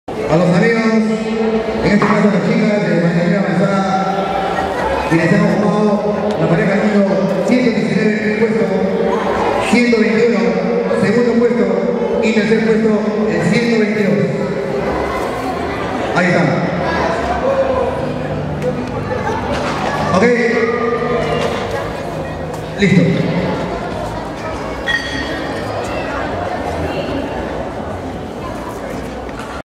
ACADEMIA "NORMA BRITO" IV CONCURSO INSTITUCIONAL MARINERA NORTEÑA 2011
Ganadores Adultos Avanzados Damas. Sabado 25 de Junio del 2011. teatro Municipal de San Juan de Lurigancho.